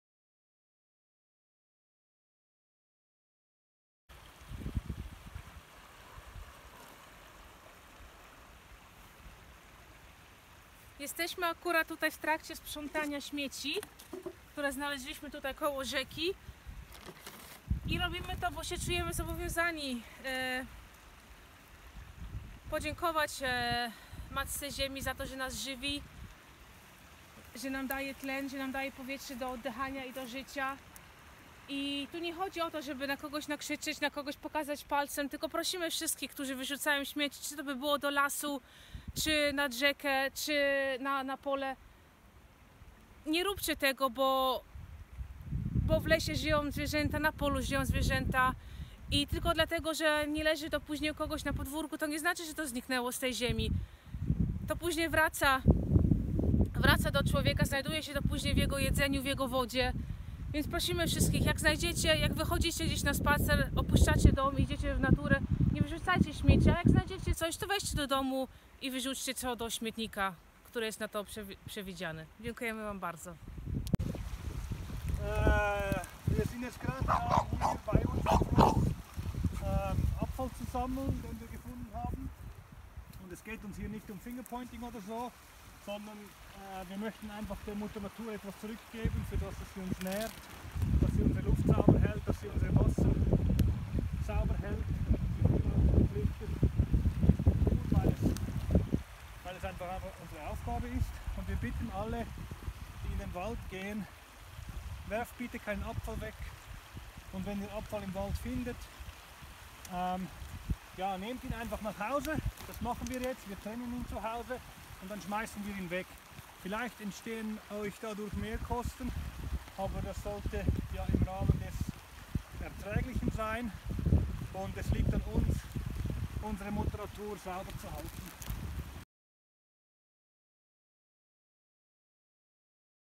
Abfall Sammelaktion Wiar
Wir sind gerade dabei, Abfall entlang des Flusses Wiar, der direkt an unser Grundstück grenzt zu sammeln. Wir fühlen uns dazu verpflichtet, Mutter Erde etwas zurückzugeben, dafür, dass sie uns täglich ernährt und uns Sauerstoff zum atmen gibt. Es geht uns nicht darum, jemanden für das illegale Entsorgen von Abfall im Wald, auf Wiesen oder Flussufer zu beschuldigen oder zu beschimpfen, sondern darum, ein Bewusstsein für Umweltschutz und unsere Verantwortung dieser Erde - unserer aller Heimat  - gegenüber zu schaffen. Es ist uns ein grosses Anliegen, den direkten Zusammenhang unseres (un)menschlichen Handelns und seine Auswirkungen auf uns selbst, unsere Familienmitglieder und Freunde und unsere Umwelt aufmerksam zu machen. Jedes Abfallstück, das illegal in der Natur entsorgt wurde, findet seinen Weg zurück zum Menschen, sei es über das Trinkwasser, die Nahrung oder die Luft. Nur weil es für das Auge nicht mehr sichtbar ist, bedeutet das nicht, dass es sich in der Luft aufgelöst hat. Wir bitten euch darum, keinen Abfall draussen liegen zu lassen und wenn ihr auf euren Spaziergängen welchen findet, diesen mitzunehmen und in den dafür vorgesehenen Abfalleimer zu entsorgen. Wir danken euch!